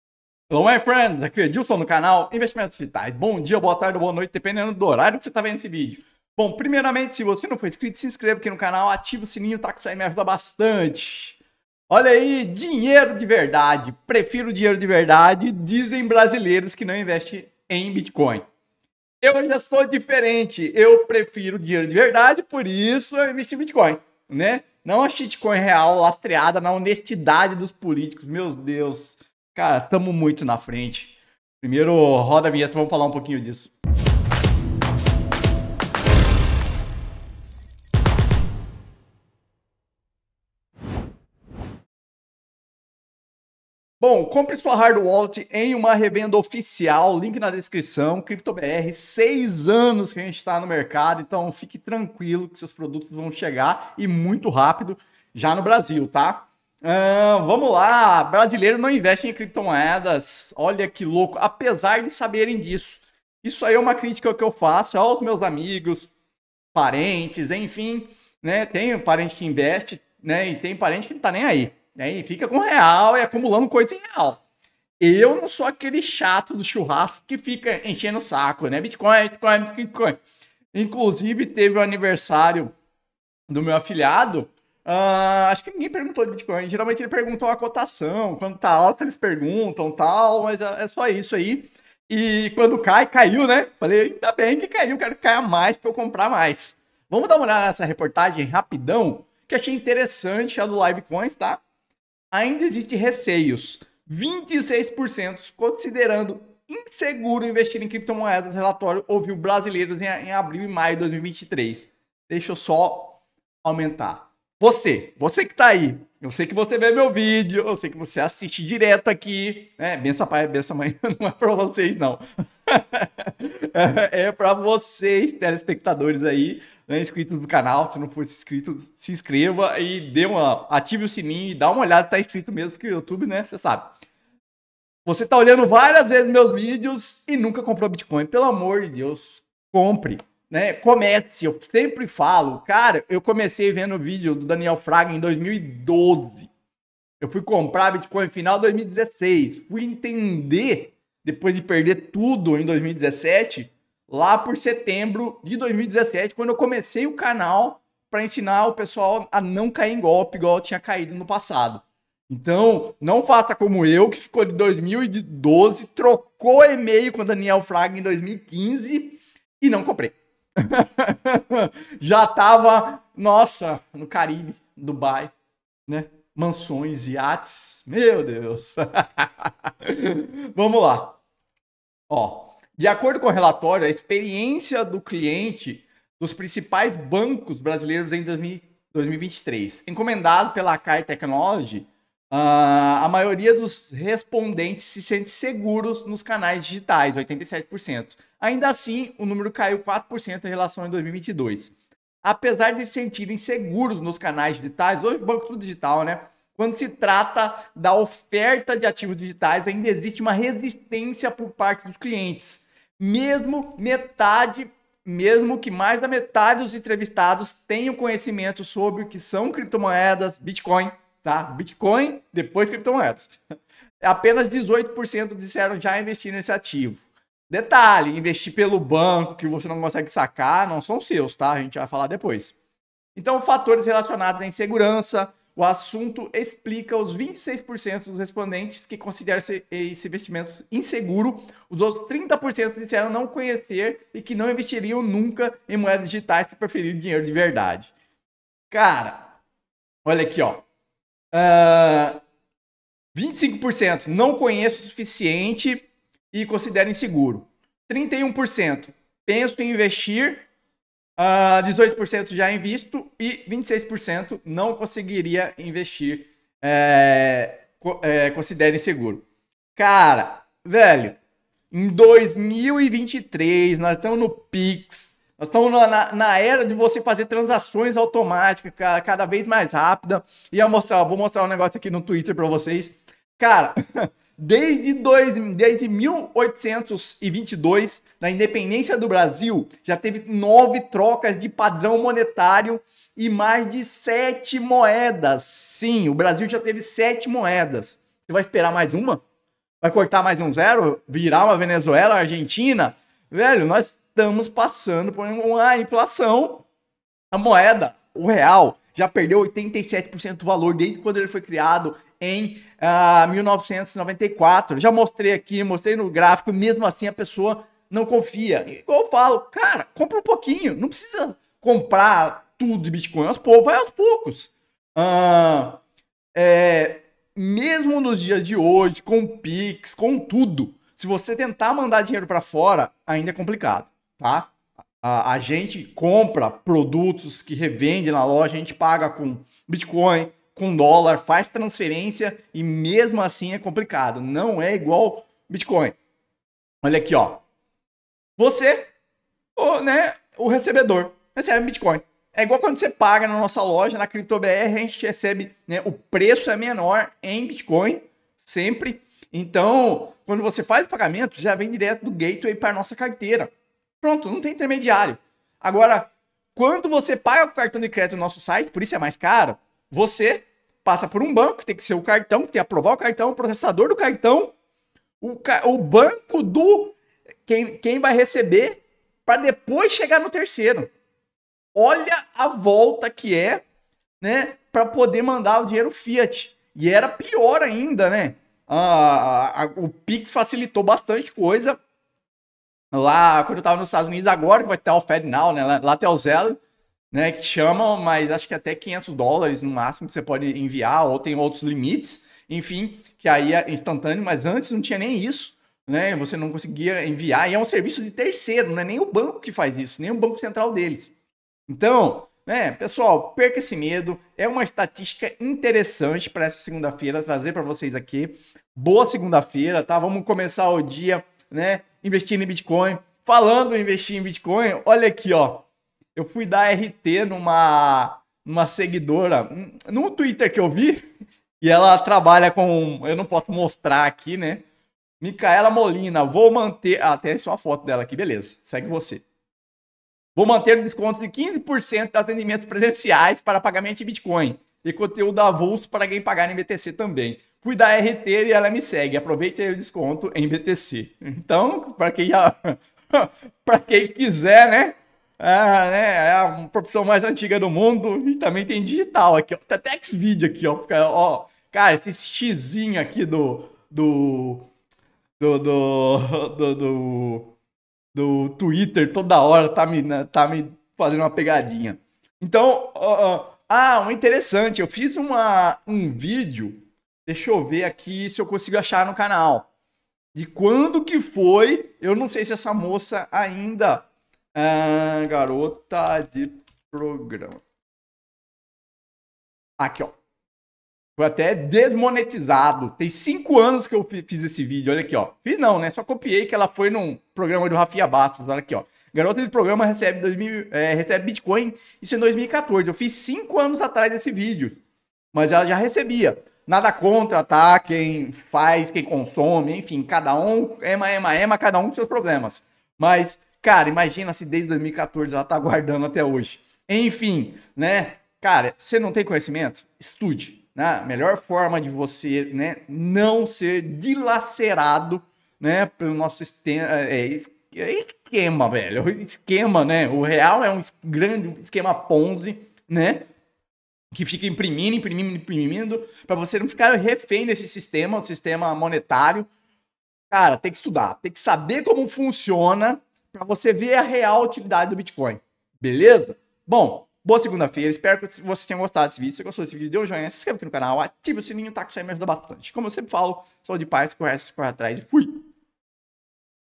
Prefiro “dinheiro de verdade”, dizem brasileiros que não investem em BITCOIN
Apesar de se sentirem seguros nos canais digitais, quando se trata da oferta de ativos digitais, ainda existe uma resistência por parte dos clientes. Mesmo que mais da metade dos entrevistados tenha conhecimento sobre o que são criptomoedas (55%), apenas 18% disseram já investir nesse ativo.
KriptoBR - Revenda Oficial América Latina
Curso Bitcoin e Criptomoedas do Básico ao Avançado